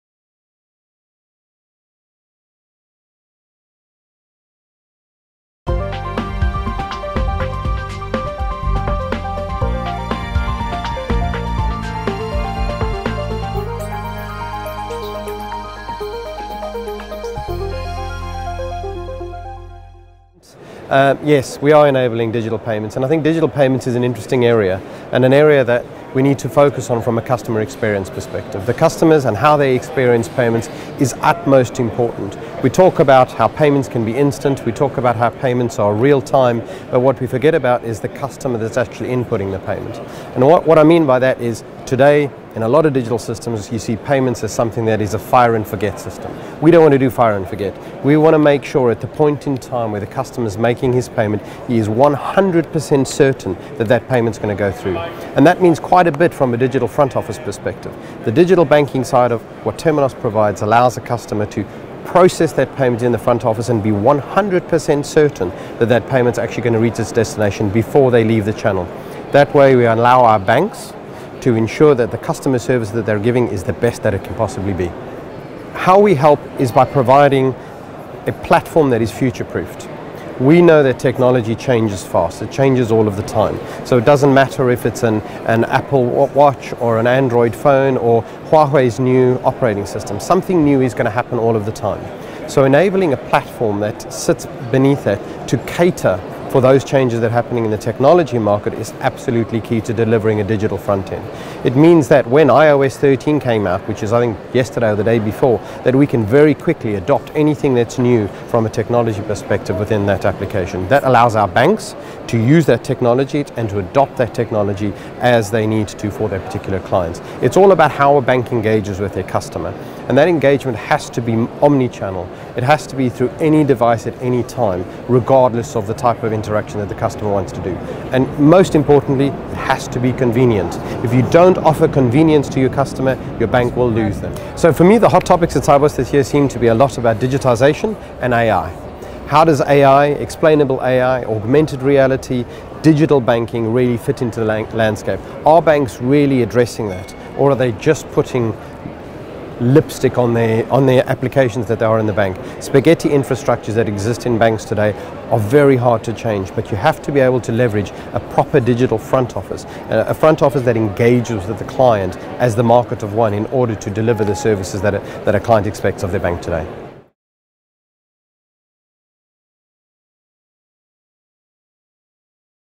Financial IT speaks with Darryl Proctor, Product Director, Payments & Universal Banking at Sibos
Financial IT speaks with Darryl Proctor, Product Director - Payments and Universal Banking at Sibos 2019